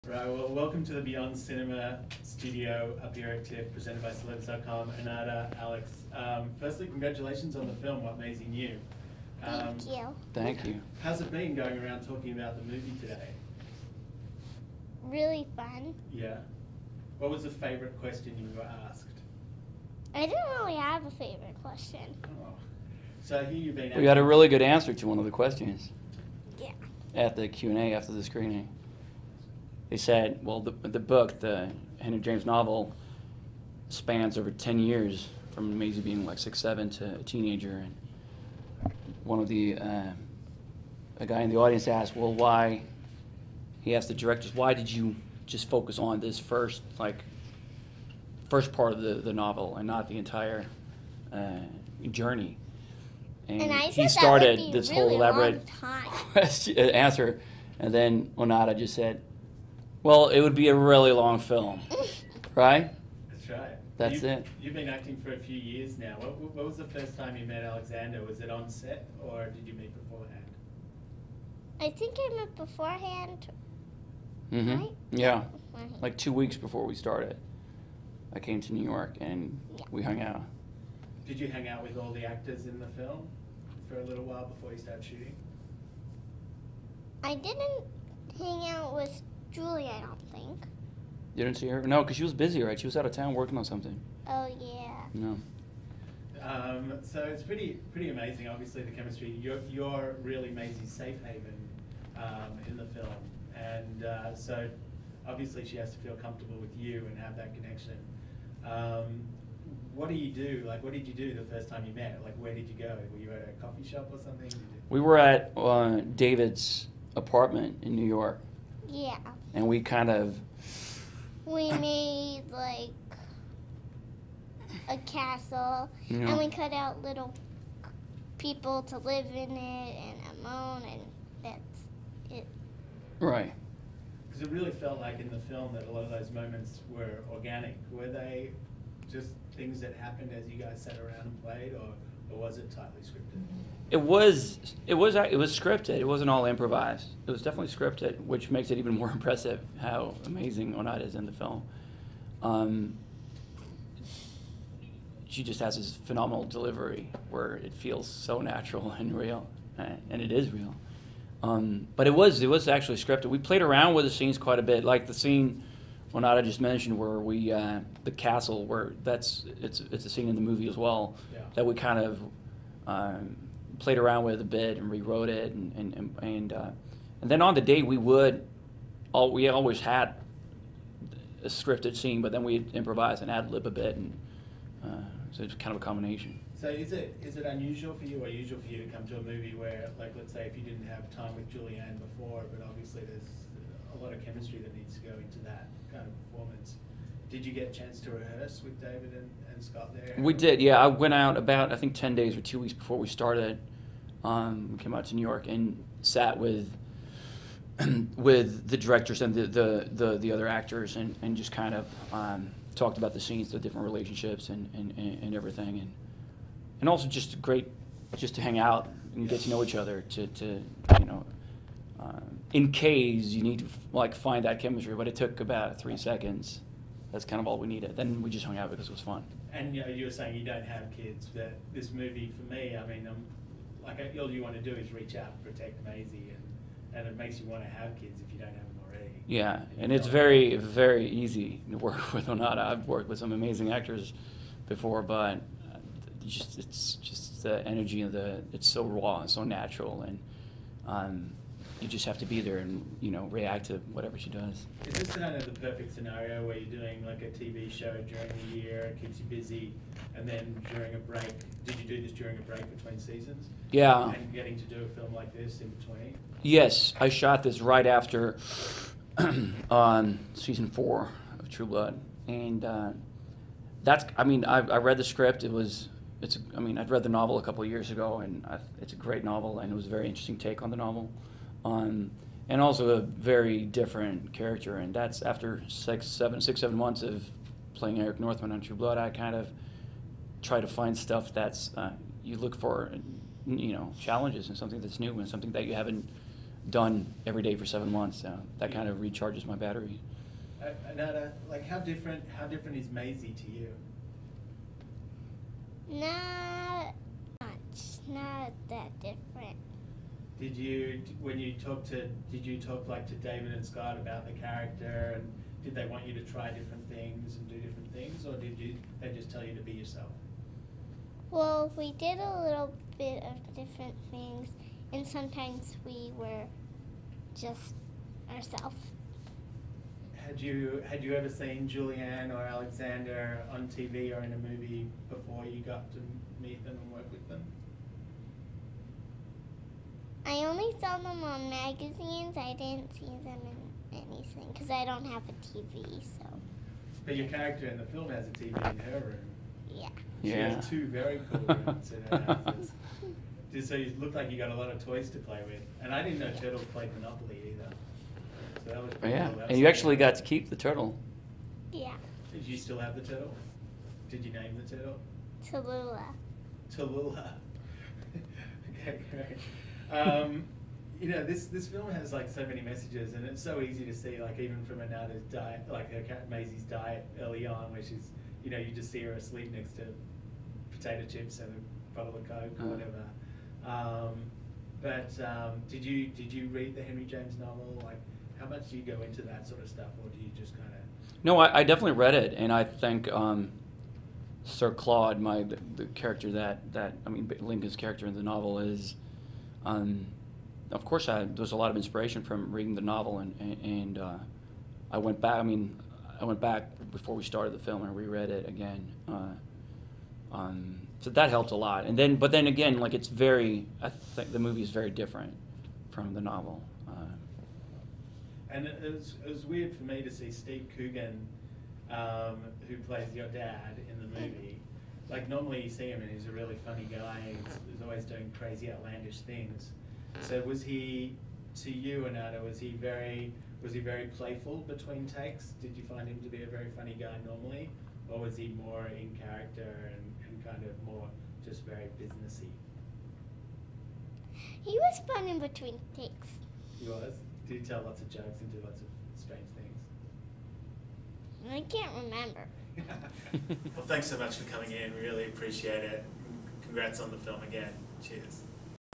Alexander Skarsgard and Onata Aprile Talk "What Maisie Knew" at TIFF 2012
Editor-in-Chief Elliot Kotek caught up with "True Blood" star Alexander Skarsgard and his "What Maisie Knew" co-star Onata Aprile at TIFF 2012. For more info on your favorite personalities head